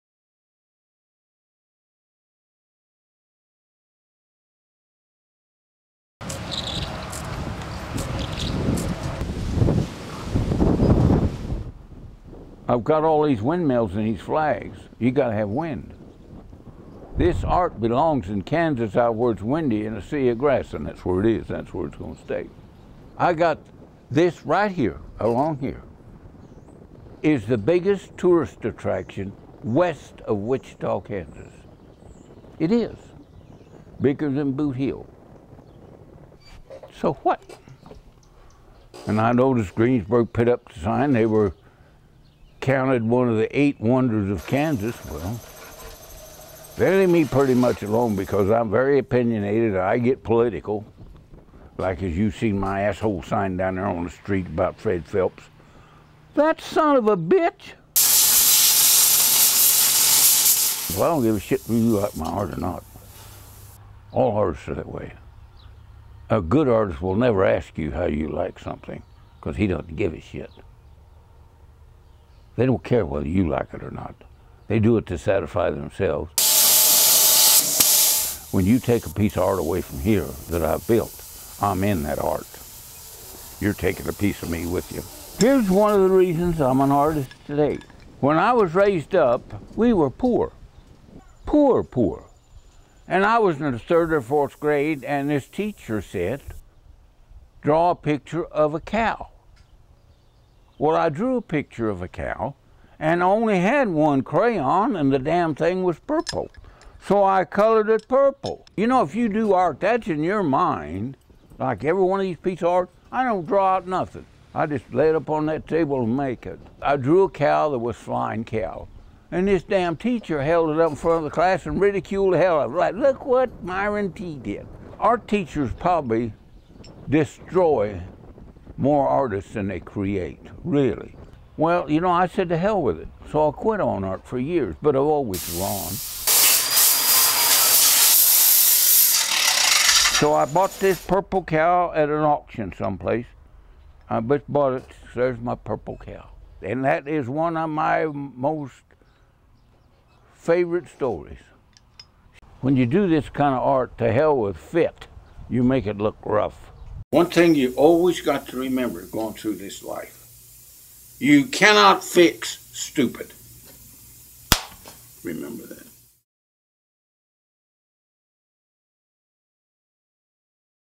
M.T. Liggett, Mullinville artist
A photo essay about M.T. Liggett an eccentric artist from Mullinville, KS.